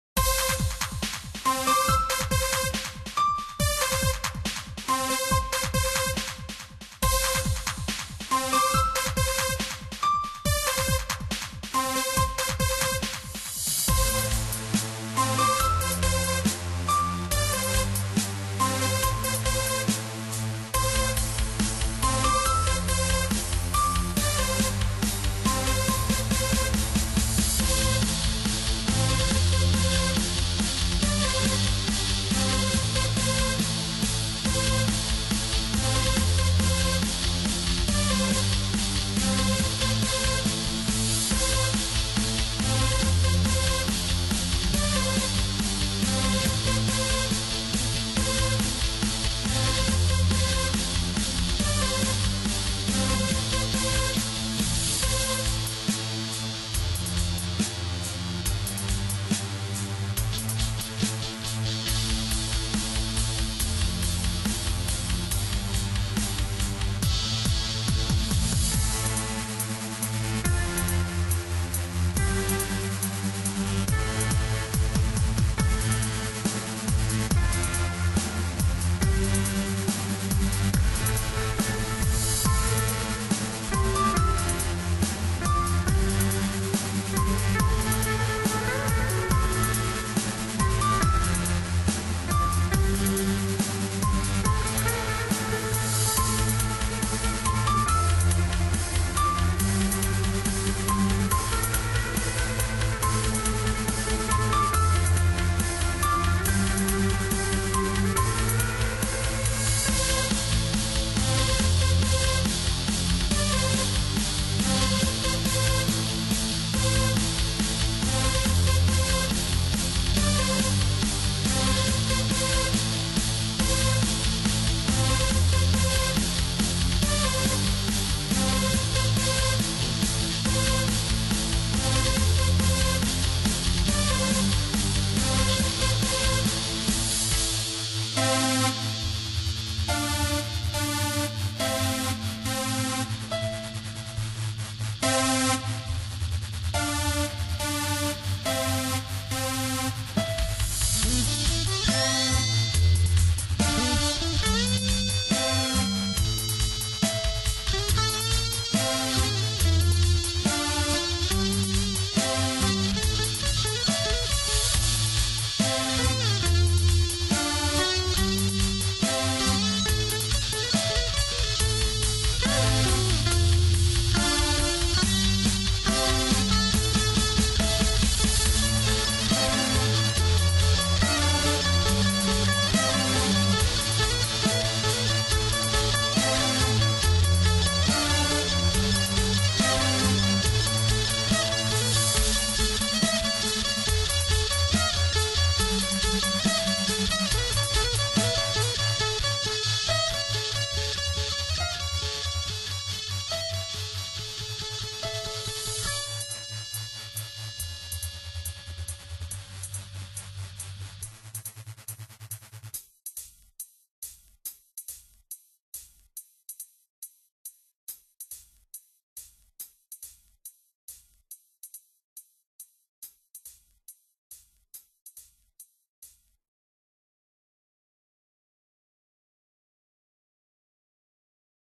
hree sided.wmv
Added a guitar track in about 20 mins today. Yes  there are mistakes, yes it needs editing. What do you think ! ??

Only had the direct line in on my laptop to record guitar and a basic effects program and a basic EQ. Do not judge the sound ? :) Just whats the idea like ?